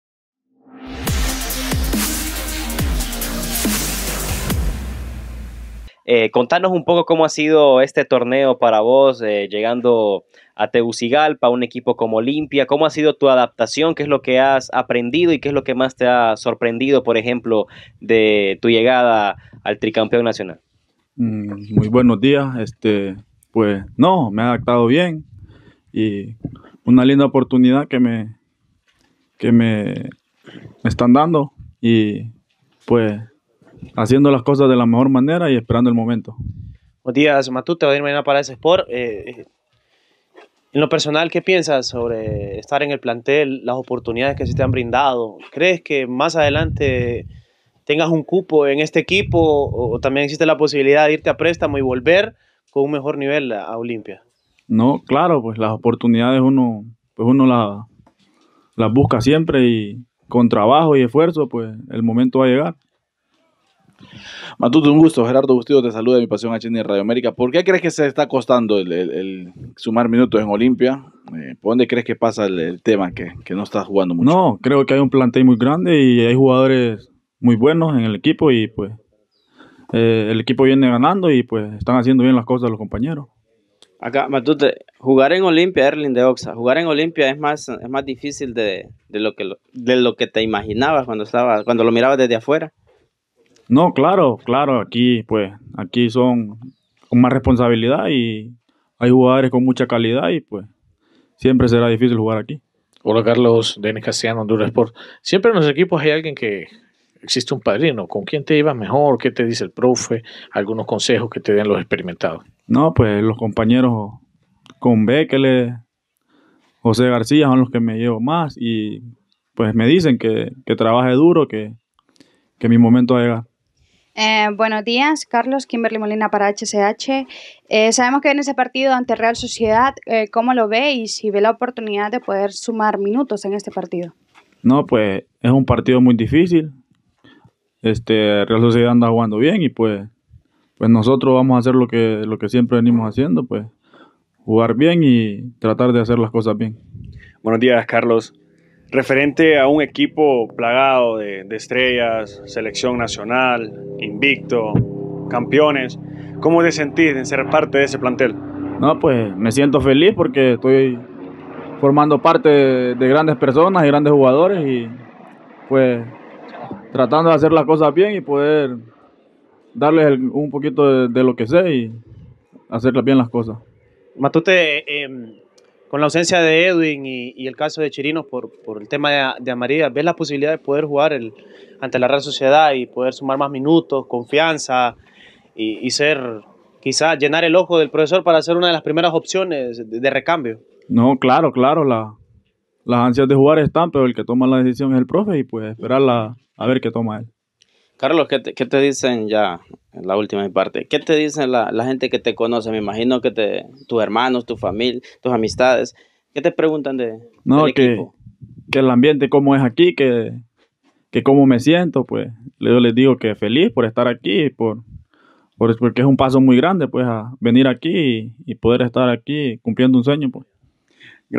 Conferencia de Prensa | Carlos Matute
Nuestro mediocampista Carlos Matute brinda conferencia de prensa en el Centro de Alto Rendimiento: José Rafael Ferrari.